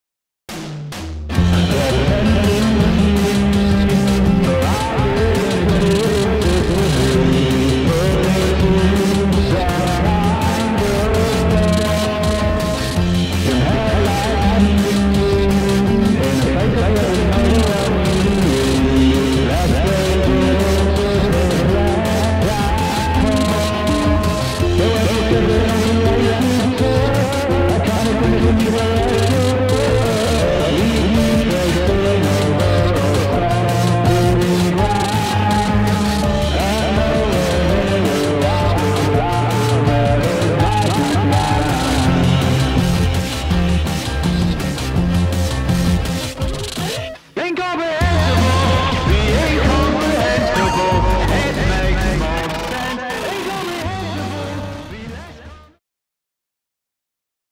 Incomprehensible T-Shirt Competition!
Here's the action video for my incomprehensible competition from summer 2021.